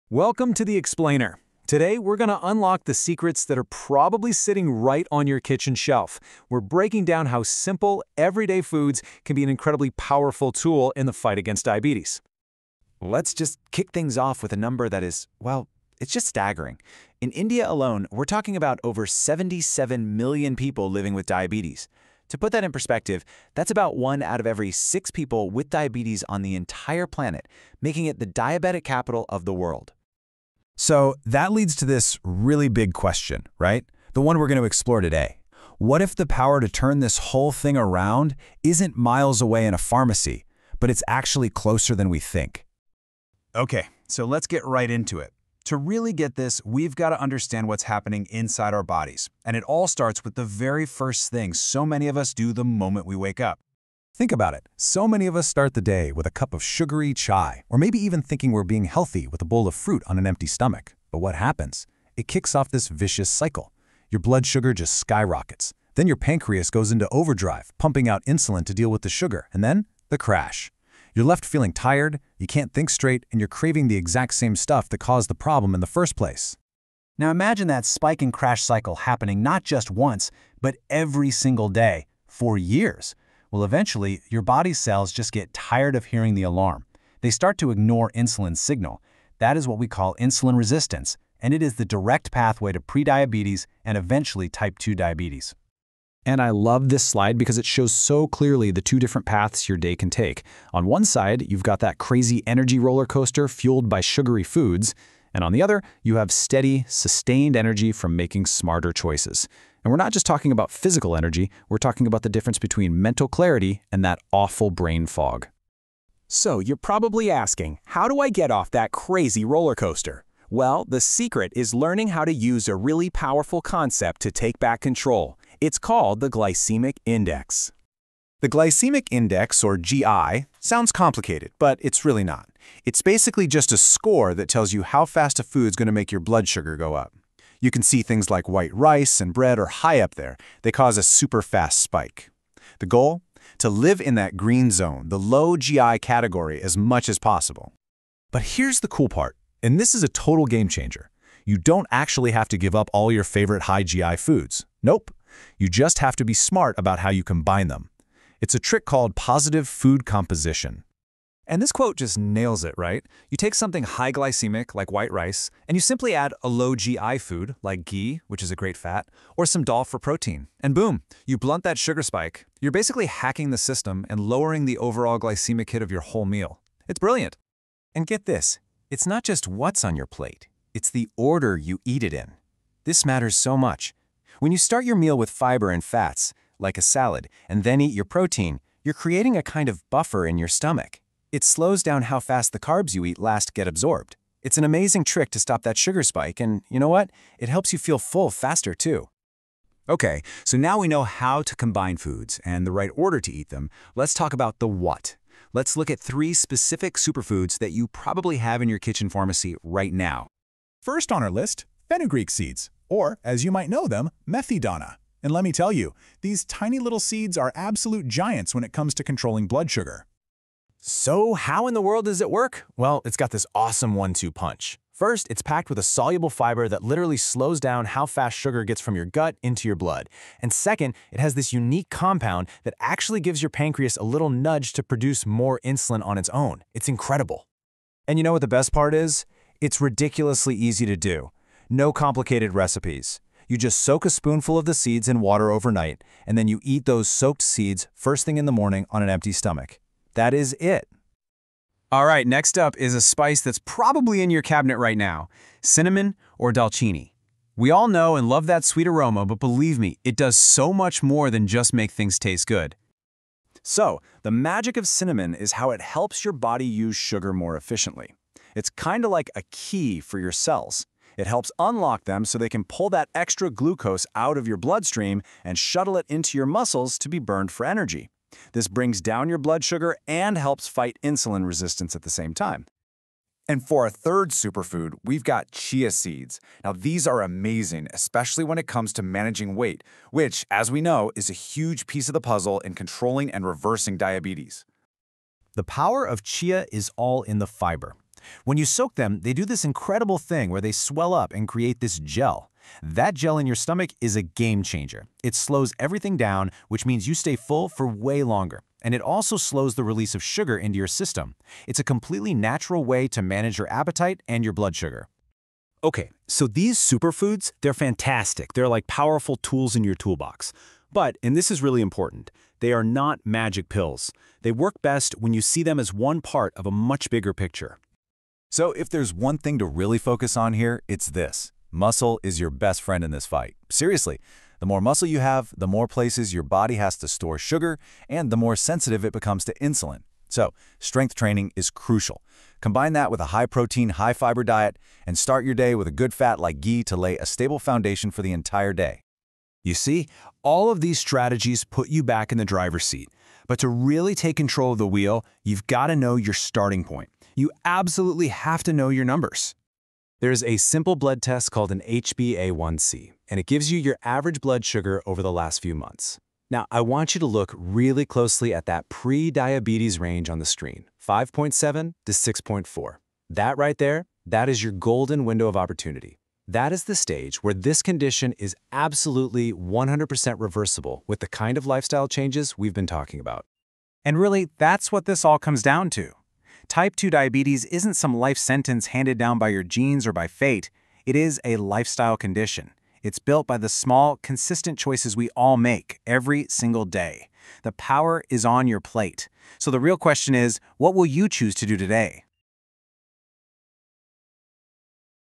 Reversing Diabetes through Superfoods!
The three superfoods—Fenugreek (Methi Seeds), Cinnamon (Dalchini), and Chia Seeds—manage blood sugar and address insulin resistance through distinct scientific and biological mechanisms, often related to fiber content, cellular signaling, and anti-inflammatory properties.
Managing blood sugar involves maintaining steady blood sugar levels throughout the day and avoiding sharp spikes and subsequent dips, which lead to cravings and eventually contribute to insulin resistance.
Here are the specific mechanisms associated with the three superfoods:
Methi Seeds (Fenugreek or Methi Dana)
Methi seeds help manage blood sugar through two key compounds: a soluble fiber and a bioactive compound.
* Slowing Sugar Absorption via Fiber:** Methi seeds contain a compound called **galactomannin**, which is a **soluble fiber**.
    *   This fiber slows down blood sugar spikes.
    *   It ensures that the pancreas secretes insulin *slowly*, which helps stabilize blood sugar levels for the entire day.
*   **Stimulating Insulin Secretion:** Methi seeds contain a unique bioactive compound called **4-hydroxyisoleucine**.
    *   This compound has the amazing property of **stimulating the beta cells on the pancreas to secrete more insulin**.
    *   This provides a natural medical and scientific effect, helping the body deal with insulin resistance in the best possible way.
Cinnamon (Dalchini)
Cinnamon works by increasing the body's sensitivity to insulin and managing oxidative stress.
*   **Increasing Cellular Uptake of Glucose:** Cinnamon contains an active compound called **cinnamaldehyde** and numerous **polyphenols**.
    *   These compounds promote **auto phosphorylation**, which increases the **uptake of sugar on the cells**.
    *   This enhanced uptake by receptors on each cell helps maintain low and steady blood sugar levels, effectively managing blood sugar spikes and insulin resistance.
*   **Promoting Glucose Transfer (GLUT4 Movement):** Cinnamon promotes the movement of a protein called **GLUT4**.
    *   GLUT4 is responsible for transferring glucose from the bloodstream to the muscles and fat cells.
    *   By transporting glucose to the muscles, it provides the muscles and body with more energy to function properly, preventing "brain fog" or blood sugar crashes.
*   **Reducing Oxidative Stress:** Cinnamon is an **excellent antioxidant**.
    *   This antioxidant property helps balance the oxidative stress in the body. For a diabetic, oxidative stress can make balancing insulin and blood sugar significantly more difficult.
Chia Seeds
Chia seeds primarily manage blood sugar and weight through their high fiber and Omega-3 content.
*   **Slowing Sugar Absorption (Natural "Ozempic-like" Effect):** Chia seeds are super rich in **fiber**.
    *   When consumed (especially when well-soaked), they **swell up in the gut** and form a **gel-like substance**.
    *   This gel naturally **slows down the absorption of the food** and the absorption of sugar from that food.
    *   This action slows down gastric emptying time, keeps an individual feeling fuller longer, and naturally reduces the quantity of food consumed, which is why it is noted to have a natural "Ozempic-like effect".
*   **Reducing Inflammation:** Chia seeds are super rich in **Omega-3 content**, specifically **alpha linolenic acid**.
    *   These Omega-3 acids help to **reduce inflammation in the body**.
    *   Reducing inflammation is crucial, as chronic inflammation can contribute to cardiovascular issues. The Omega-3 content also makes the seeds cardioprotective, protecting the heart and blood vessels by maintaining the elasticity of the blood vessel walls.
These mechanisms—slowing down gastric emptying, stimulating insulin production, increasing cellular glucose uptake, and reducing inflammation—are key to managing diabetes and reversing pre-diabetic conditions, which are often characterized by high blood sugar levels and chronic insulin resistance.
Note ; please consult your physician before making decisions based on this video information. This video is for knowledge sharing only and not for your treatment